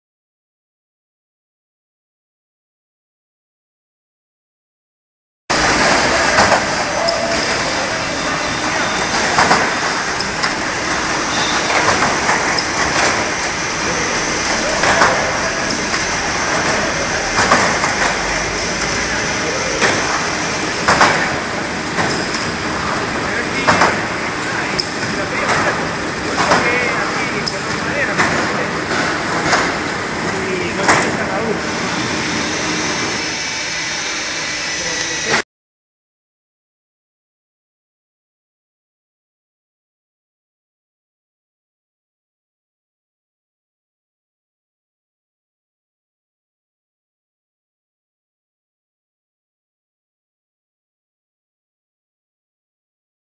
Case Orientation for Palletizing Application (G20 A01SR Scara)
EPSON G20-A01SR ceiling mount scara robot grabs cases of liquids and re-orients them in the correct direction for a downstream palletizing operation.